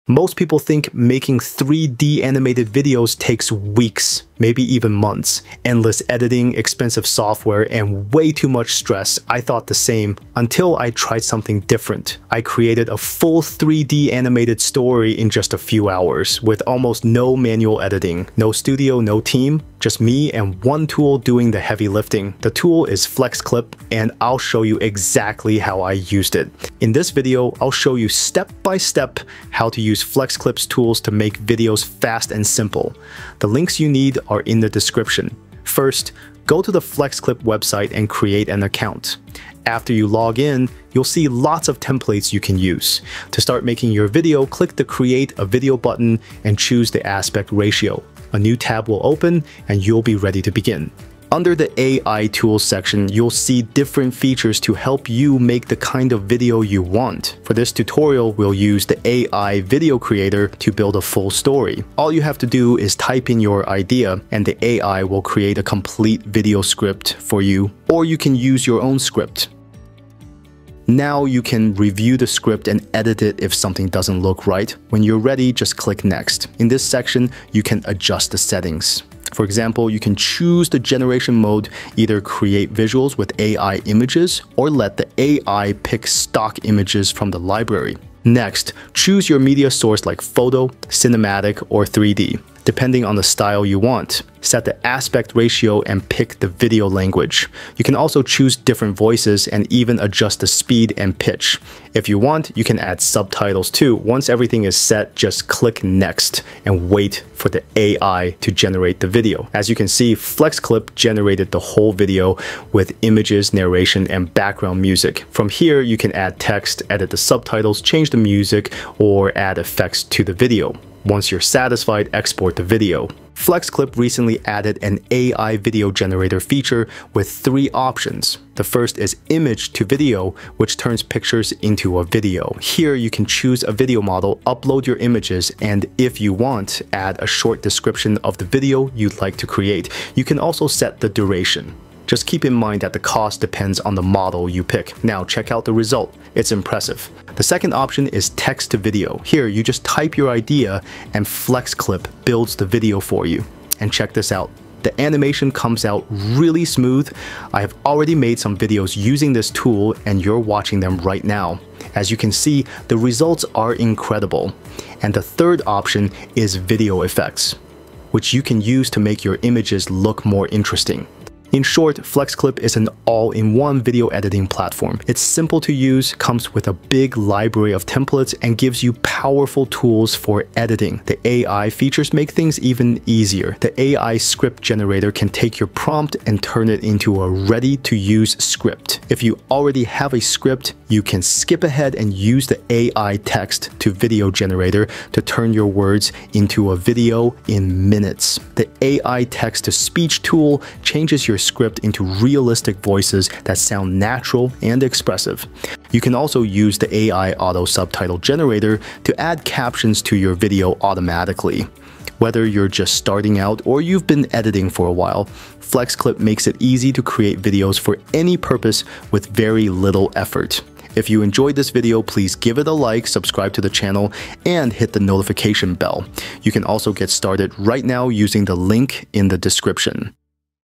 I Made a 3D Animated Story in Minutes With Just ONE Tool”
Want to create 3D animated videos without expensive software or a big team? In this tutorial, I’ll show you how to make a 3D animated story, short film, or kids video using just one AI tool — fast and simple!
Most people think making 3D animation takes weeks, endless editing, and costly software. But with this tool, you can create:
3D animated explainer videos
3D animated short films
Kids 3D animated stories
3D product or presentation animations

In this step-by-step tutorial, I’ll walk you through how to use FlexClip’s AI video generator, text to video, and image to video features. You’ll learn how to create consistent animated characters, add realistic AI voiceovers, subtitles, background music, and export your project in minutes.

Whether you’re a beginner or an experienced creator, this guide makes AI animation easy, fun, and professional.